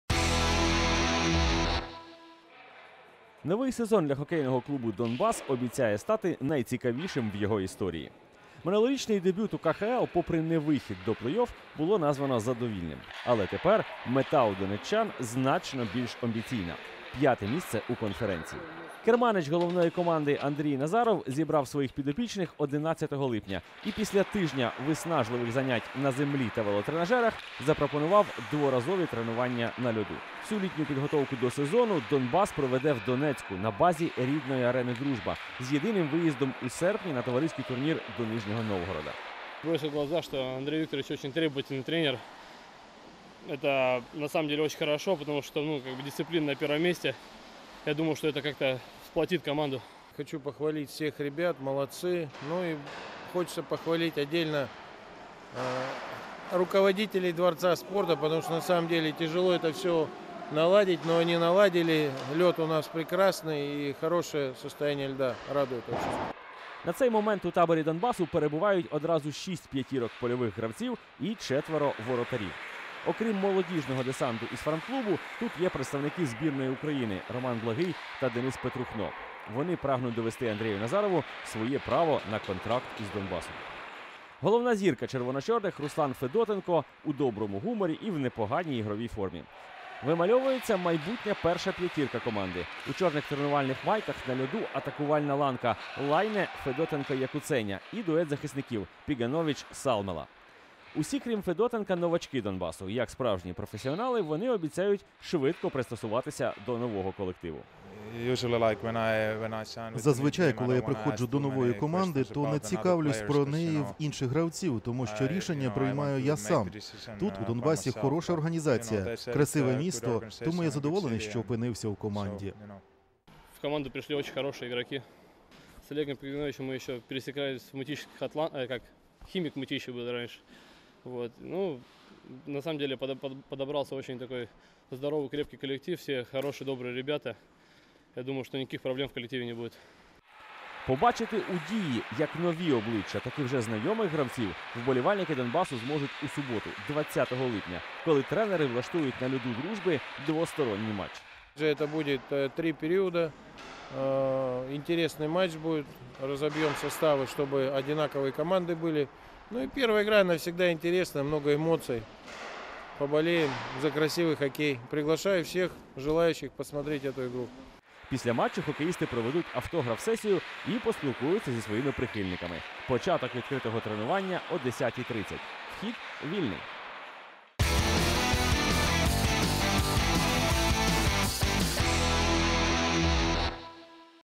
Первый в новом сезоне выход на лед
Про очертания первой пятерки и атмосферу в целом.
Андрей Назаров доволен как первым выходом на лед, так и его состоянием.